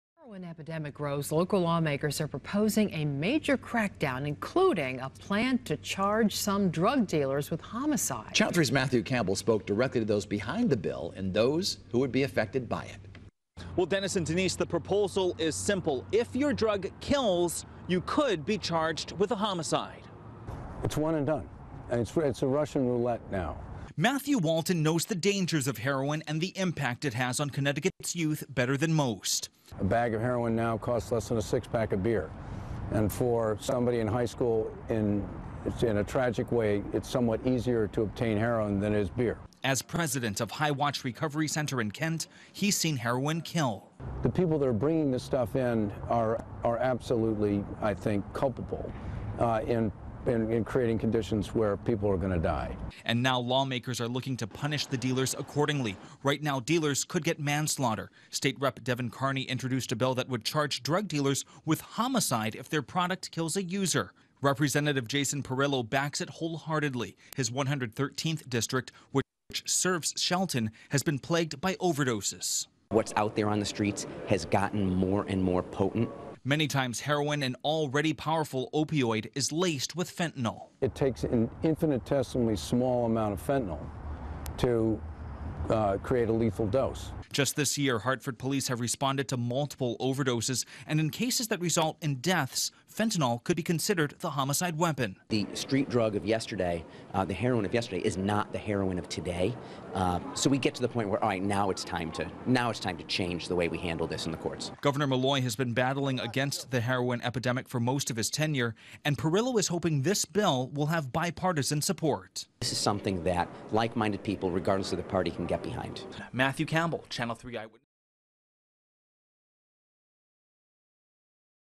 Rep. Perillo talks bigger penalties for opioid dealers on WFSB 3
Rep. Jason Perillo (R-113) talks about legislation proposed this session he is supporting which will make those who deal fatal doses of opioids liable for murder charges. WFSB Channel 3 interview, aired Jan. 23, 2017.